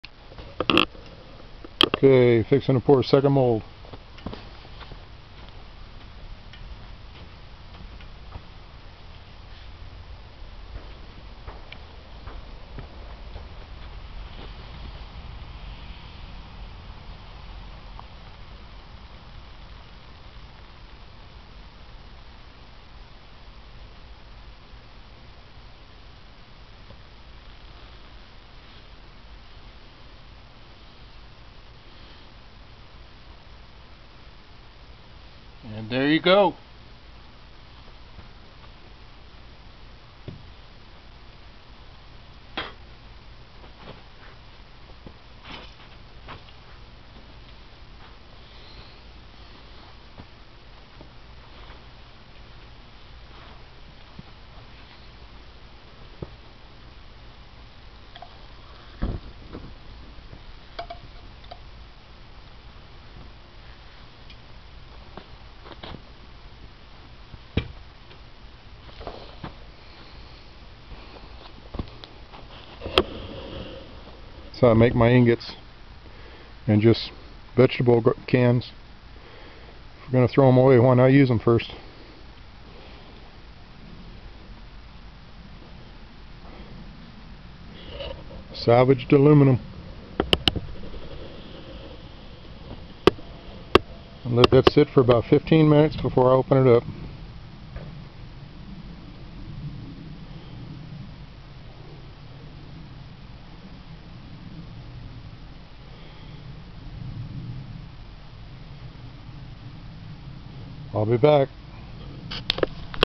3d printer print plate Aluminum casting
Casting an aluminum print plate for my Prusa Mendel 3d printer, taking you through the whole process.